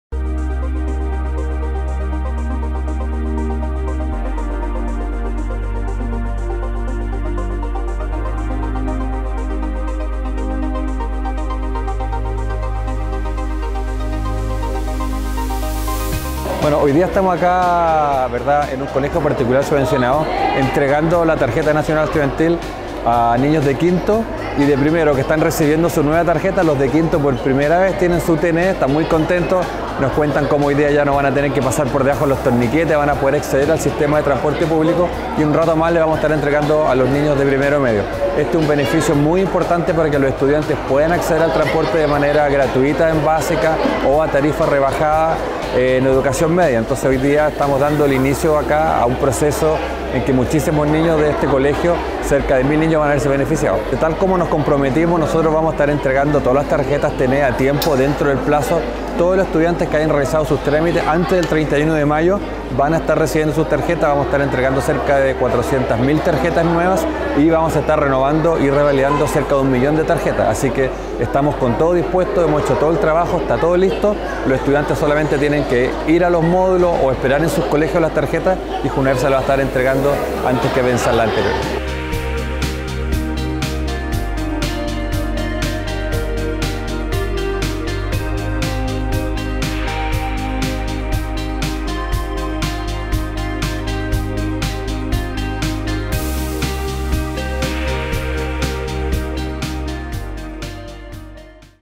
Entrega TNE 2018 Colegio Manuel José Irarrázaval
El Director Nacional de Junaeb, Jaime Tohá, participo de la entrega de la Tarjeta Nacional Estudiantil en el colegio Manuel José Irarrázaval de la comuna de Santiago. En el establecimiento 132 estudiantes de quinto básico y primero medio, recibieron su TNE 2018, que les permitirá acceder al transporte público con una tarifa rebajada.